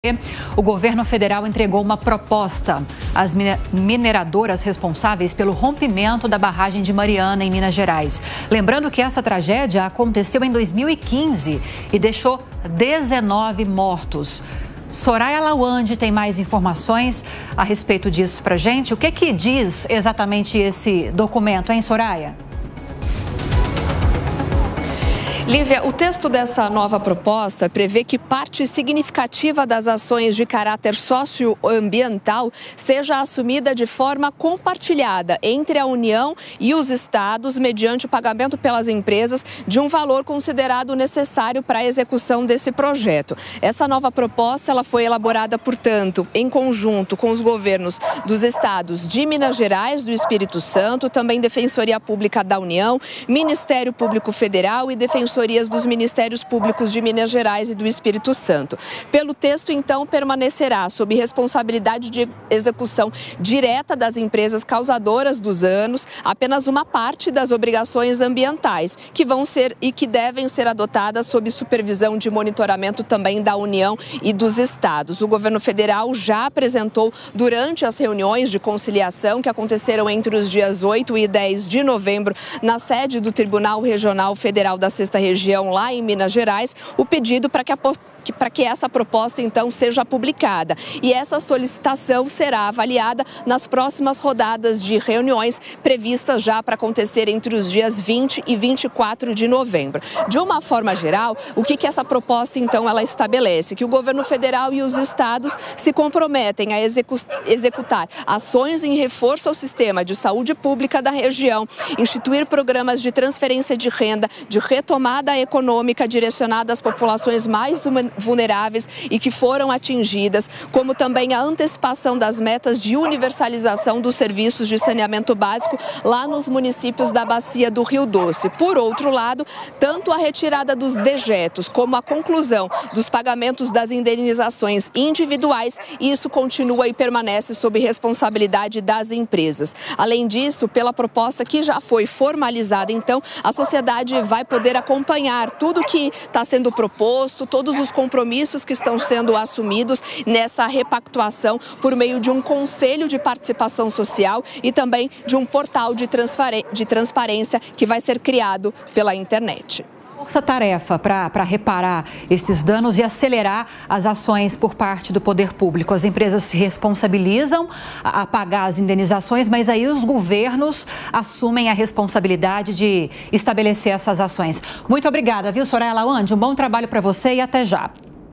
Governo federal propõe assumir medidas para reparar desastre em Mariana, MG
O governo federal entregou nesta sexta (17) uma proposta às mineradoras responsáveis pelo rompimento da barragem de Mariana, em Minas Gerais. As ações serão assumidas mediante o pagamento das mineradoras.